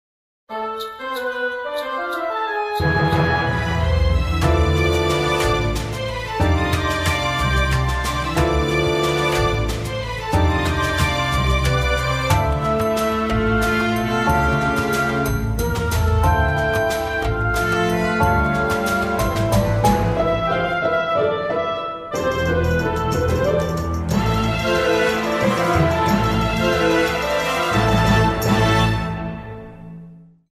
Spot Ballet Flamenco de Andalucia. Metáfora 2012
Sport de Ballet Flamenco de Andalucia. Espectaculo en el año 2012 "Metáfora". Instituto Andaluza del Flamenco. Sevilla